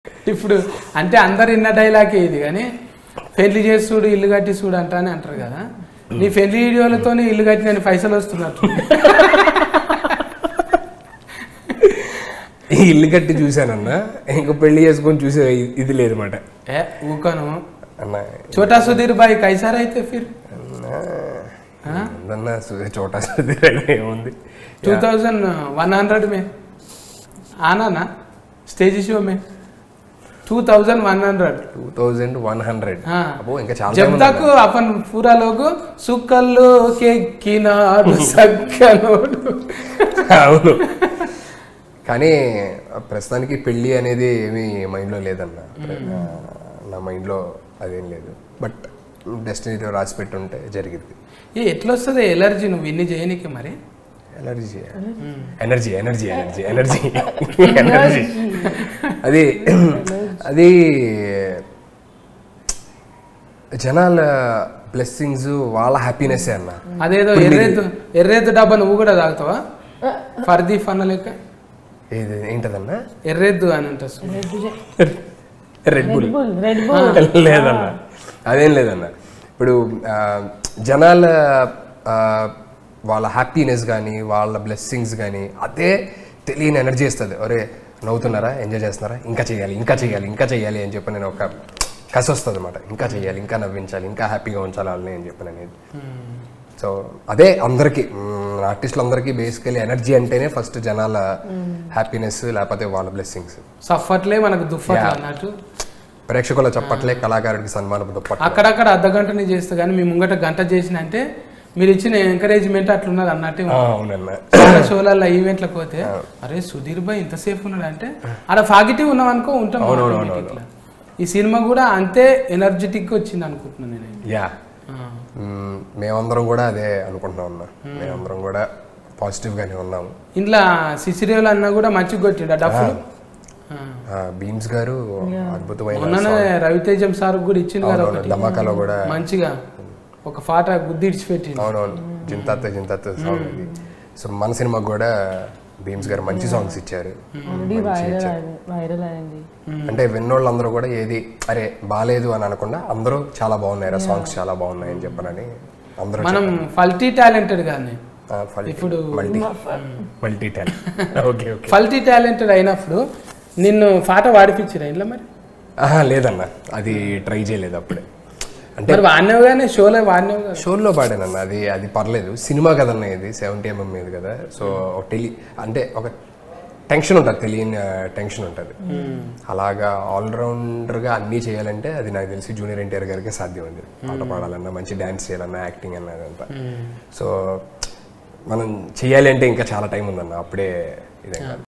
Sudigali Sudheer Unexpected Answer To Bithiri Sathi Question Over His Marriage | Daily Culture
Sudigali Sudheer Unexpected Answer To Bithiri Sathi Question Over His Marriage